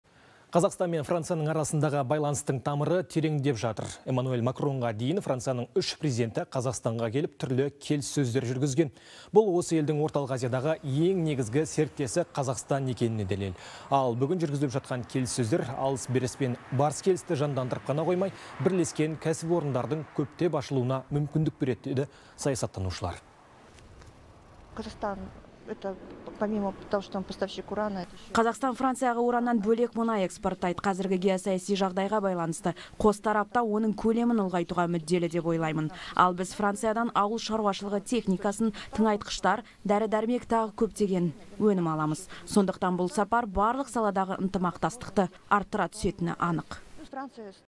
Қазақстан Орталық Азия аймағында Францияның негізгі серіктесі
Қазақстан мен Францияның арасындағы байланыстың тамыры тереңден жатыр. Эмманюэль Макронға дейін Францияның 3 президенті Қазақстанға келіп, түрлі келіссөздер жүргізген. Бұл осы елдің Орталық Азиядағы ең негізгі серіктесі Қазақстан екеніне дәлел. Ал бүгін жүргізіліп жатқан келіссөздер, алыс-беріс пен барыс-келісті жандандырып қана қоймай, бірлескен кәсіпорындардың көптеп ашылуына мүмкіндік береді, дейді саясаттанушылар.